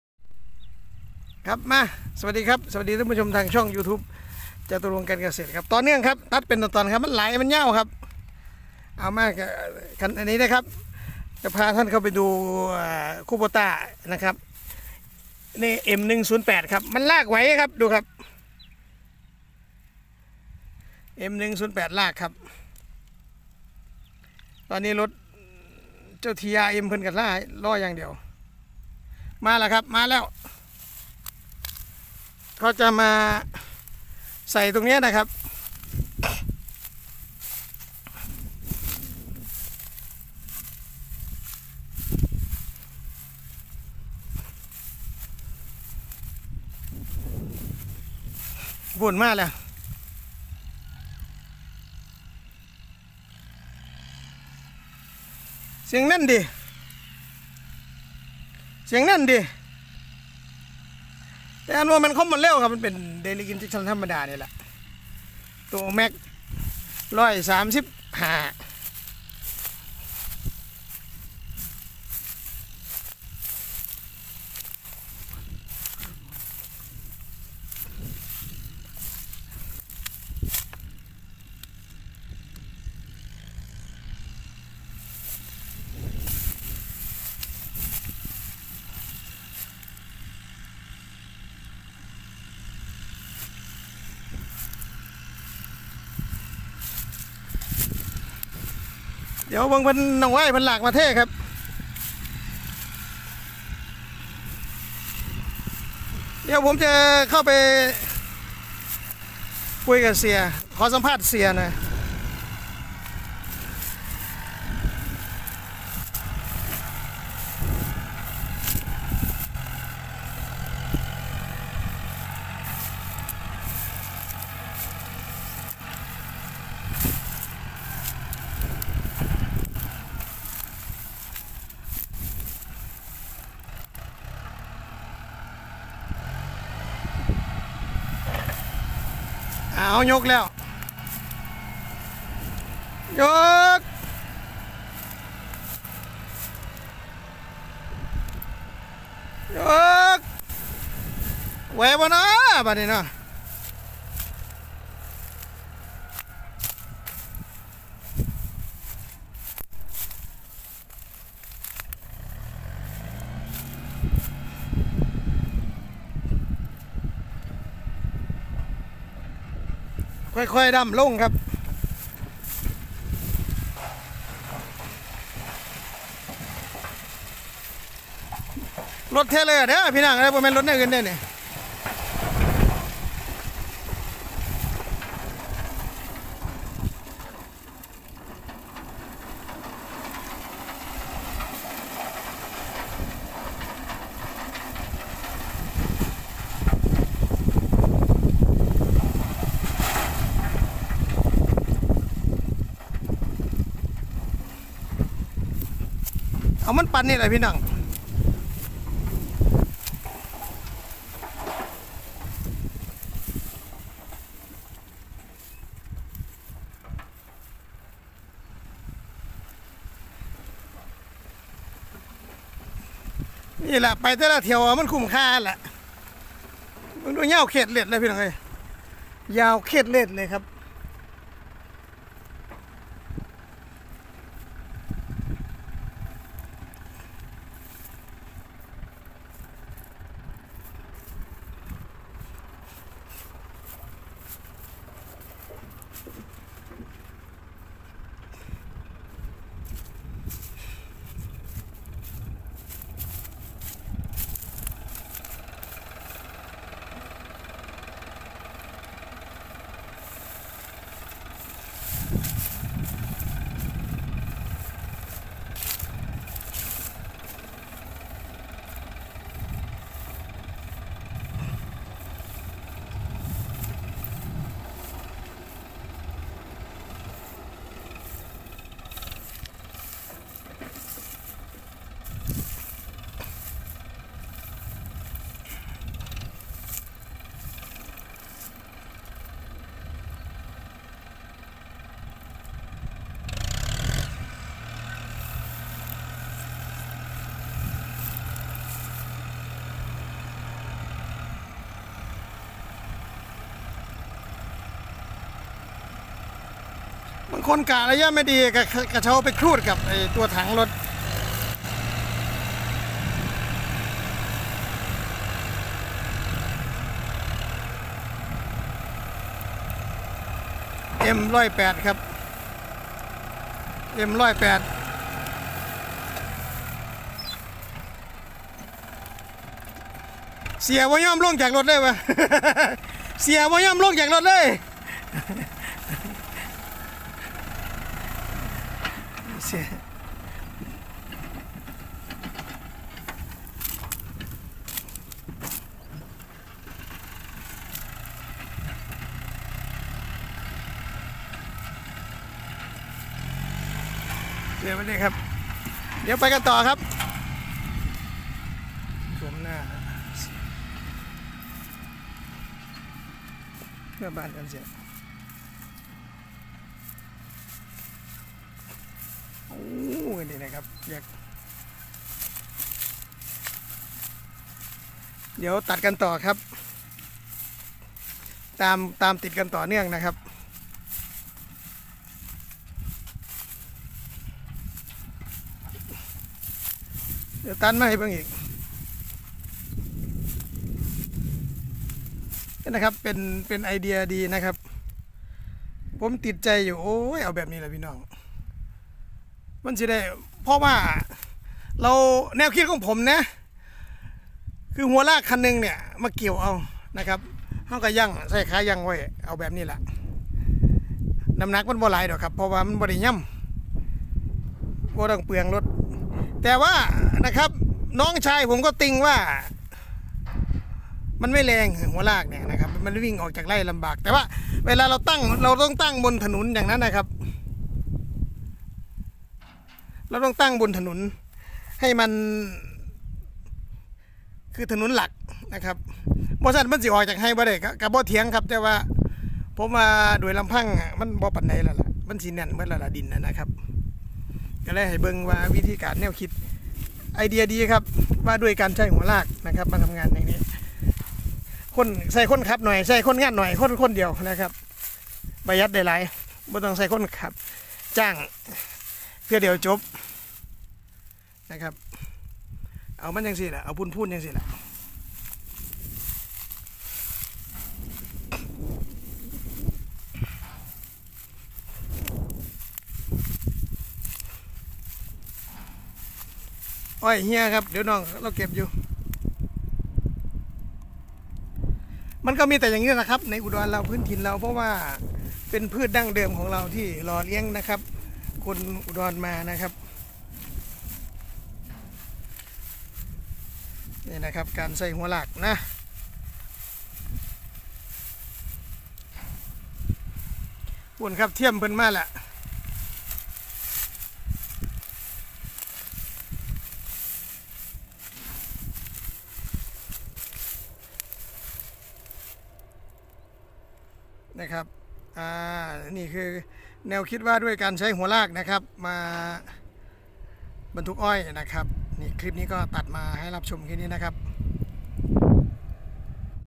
การนำรถหัวลากมาใช้บรรทุกอ้อย#ชาวไร่อ้อย
ไปดูแนวคิดการนำรถหัวลากมาช่วยบรรทุกอ้อยในไร่ครับ ไปได้เยอะ ประหยัดแรงงาน